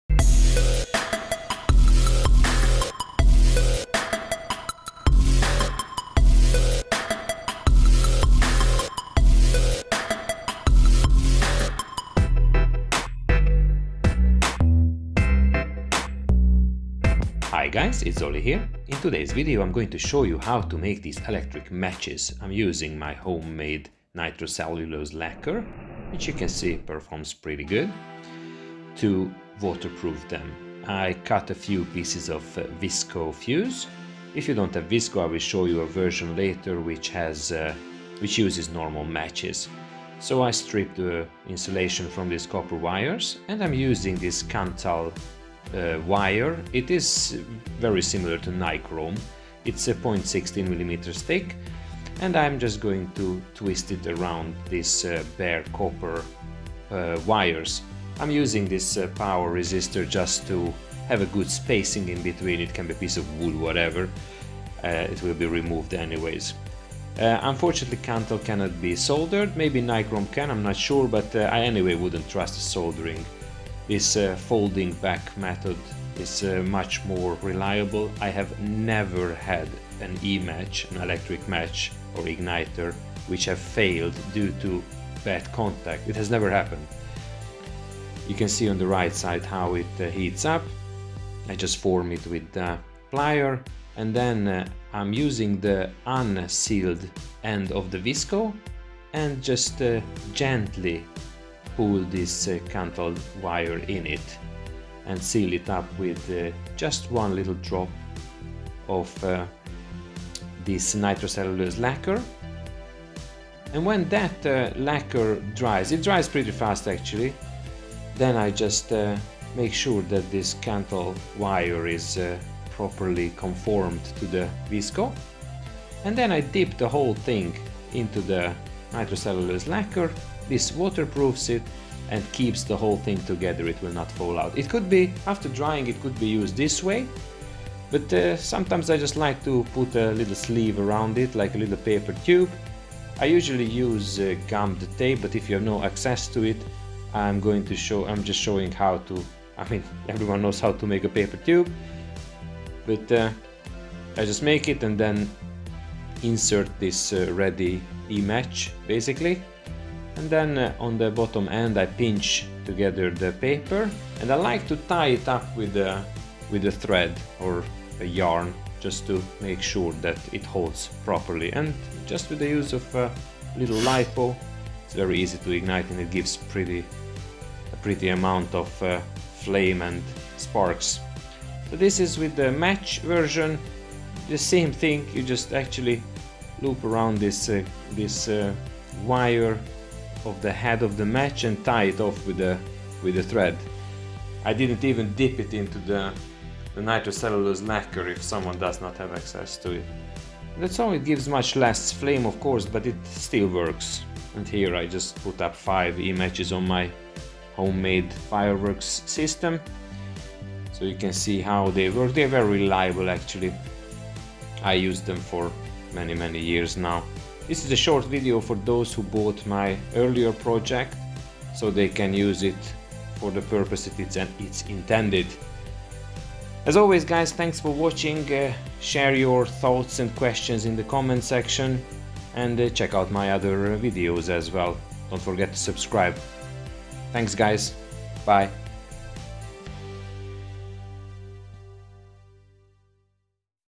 Home made E-match, electric fireworks igniter.
Hi!

In this short vid I show how I make my E-matches to make my fireworks experience safer and more fun.